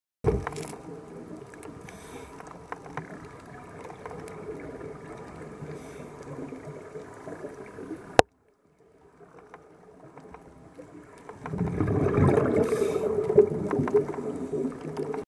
ambiance mer rouge croisière Saint Johns
La magie de la mer rouge : anthias et mérou  ( par JLH)
fin décembre 2019